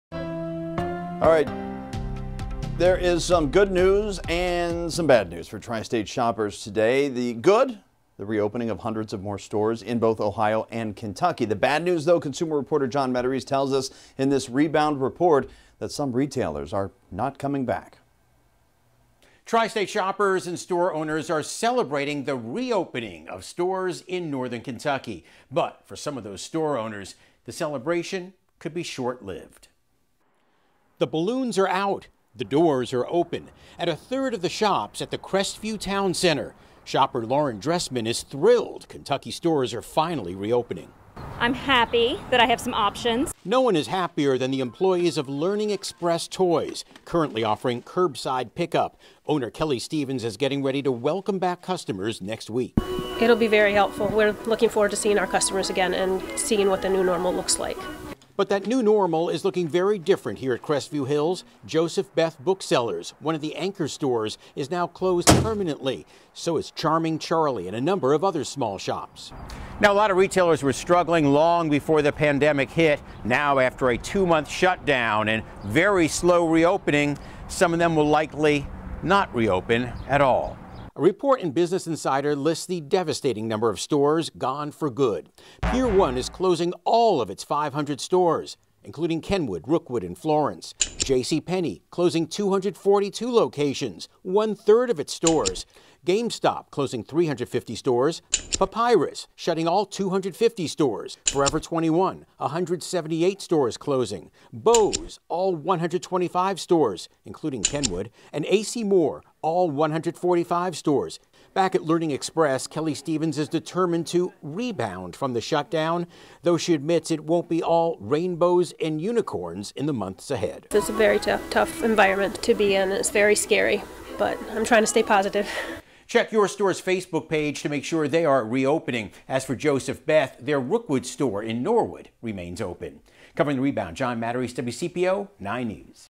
DWYM: Major stores closing
TRI STATE SHOPPERS ARE CELEBRATING THE REOPENING OF STORES IN KENTUCKY.              BUT FOR SOME STORE OWNERS, THAT CELEBRATION COULD BE SHORT  LIVED.  A  LOT OF RETALERS WERE STRGGLING LONG BEFORE THE PANDEMIC HIT.            NOW AFTER A TWO MONTH SHUTDOWN, AND VERY SLOW REOPENING, SOME OF  THEM WILL  LIKELY NOT REOPEN AT ALL.